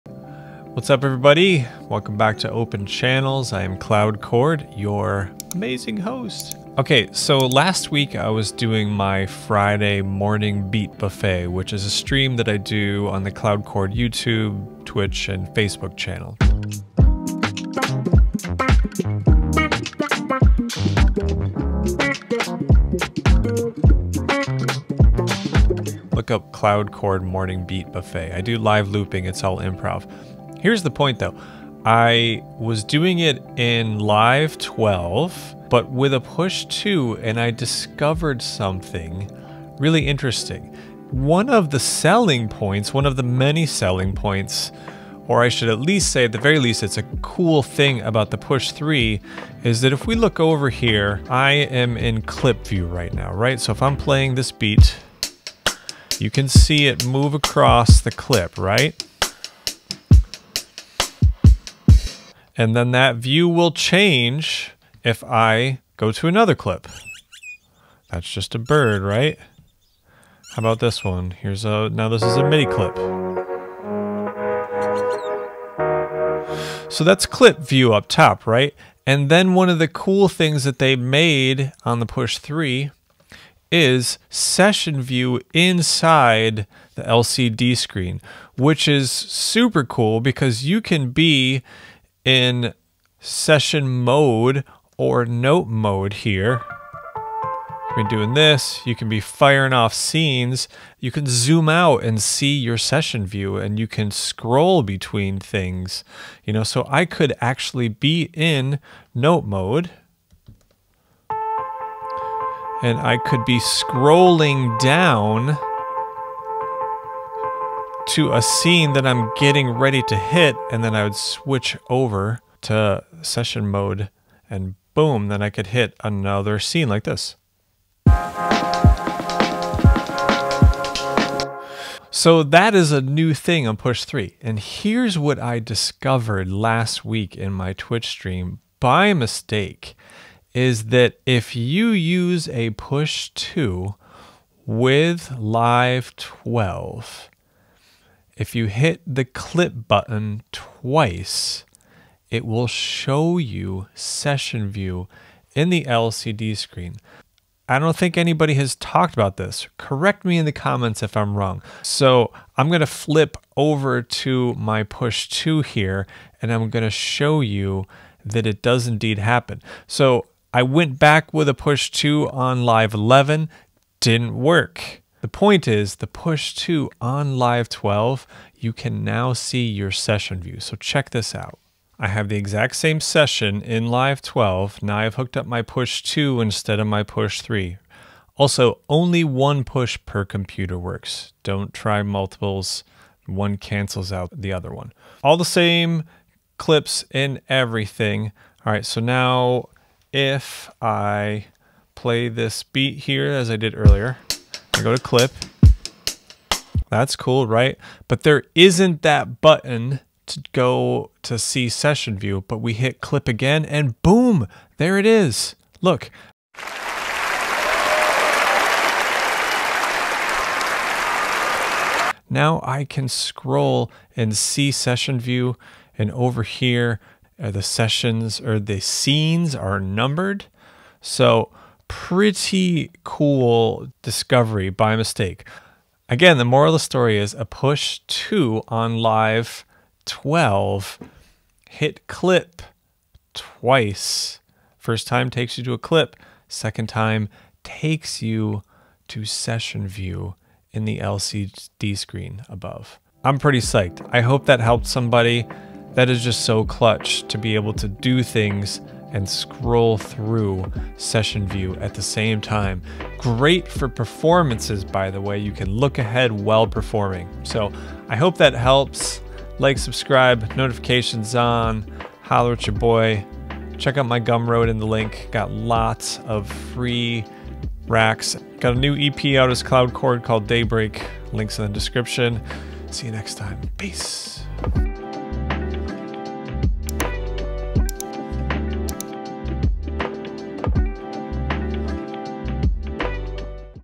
Push 2 in Live 12 Update (Session View)
Here's a Push 2 trick that very people have discovered in Live 12.